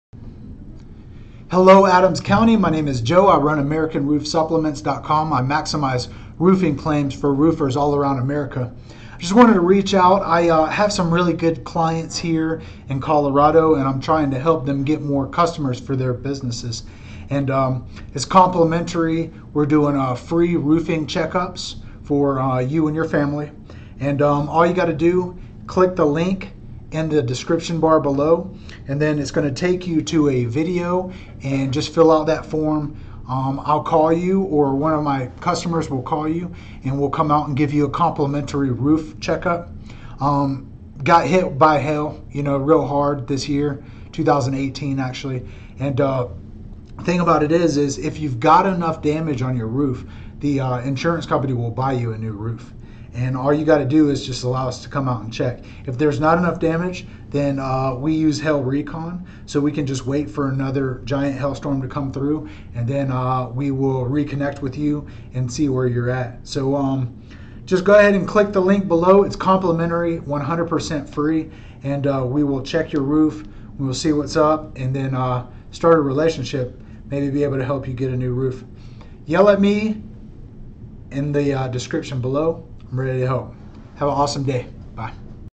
Best Roofing Company in Adams County Colorado
So I want to be able to help you find the right contractor in Adams County Colorado. I work with hundreds of roofing contractors to maximize their clams by doing roofing supplements for their companies, and I’m just trying to get them more customers for their businesses. It’s one of those I’ll scratch your back if you scratch my back kind of things.

Click the link to my website, and it will take you to a form go ahead and fill that out I will call you, and I will set up an appointment with one of my trusted roofing contractors in Adams County Colorado.

You might have wind damage, or you might have hail damage, and you might not even jump up on your roof to check. I know for sure back in the day when I wasn’t a roofer there would be no way that you would ever find me checking my roof.

Thank you so much for checking out my video on Best Roofing Company in Adams County Colorado.